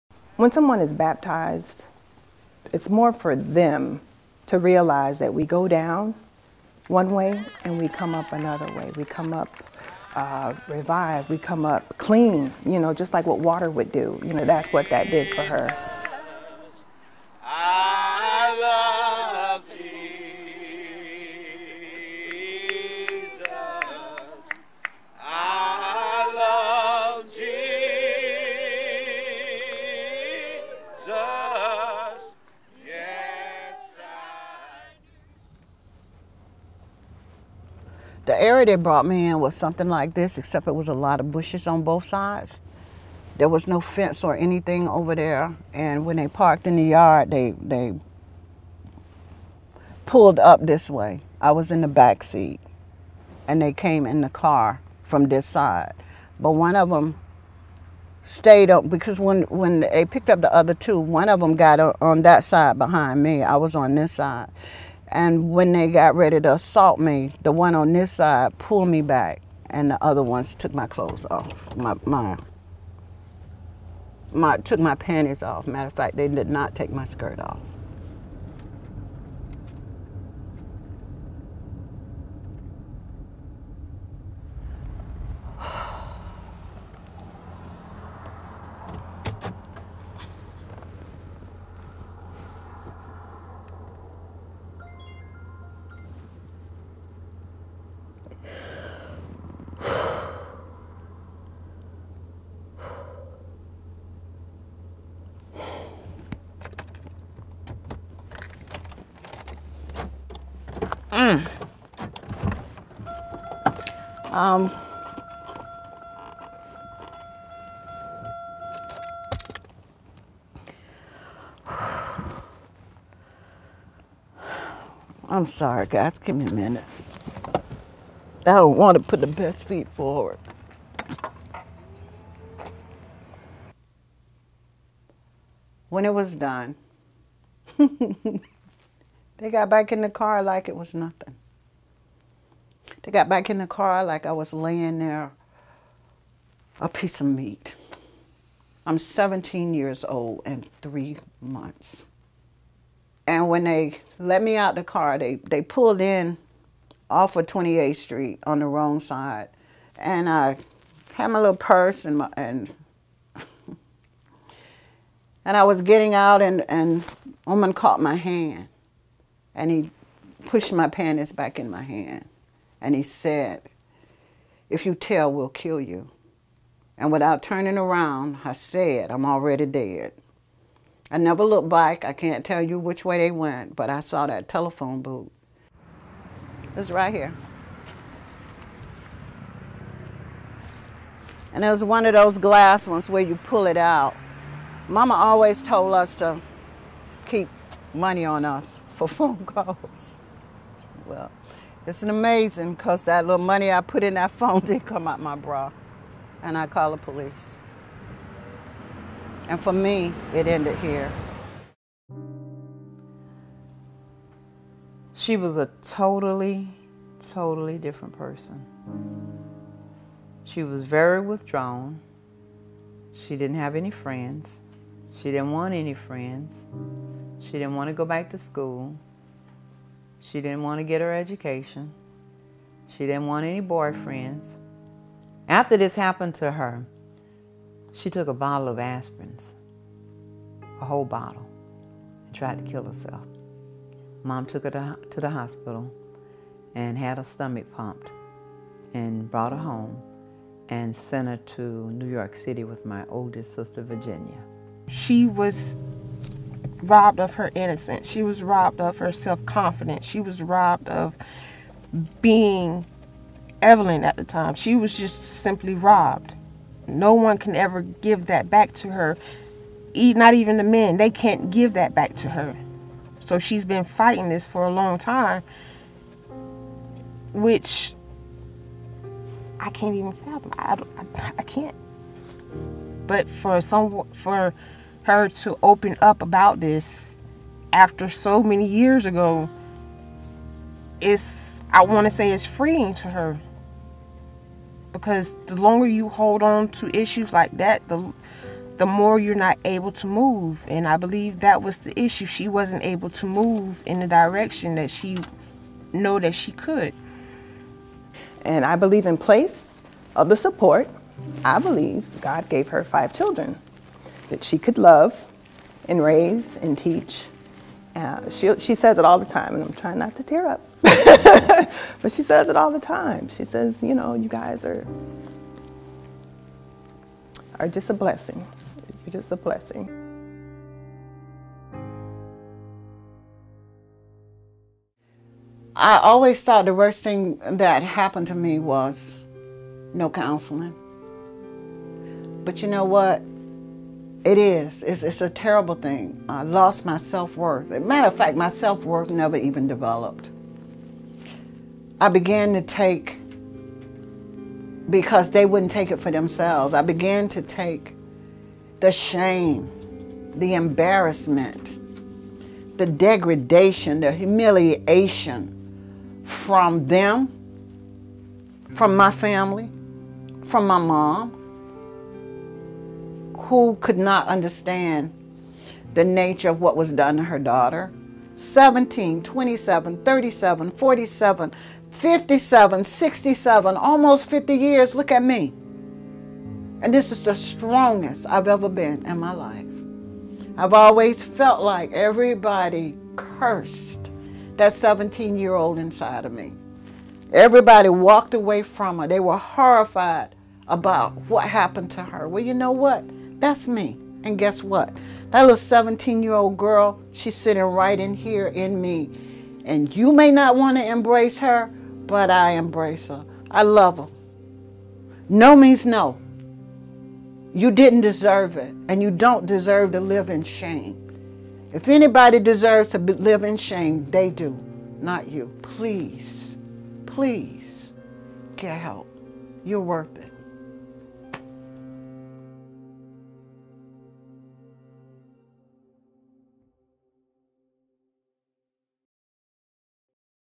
Gang raped at 17. Therapy at 65.
Evelyn Robinson was raped by four men in St. Petersburg, FL in 1969. She was 17 years old. Forty-eight years after being attacked, she searches for a way to cope.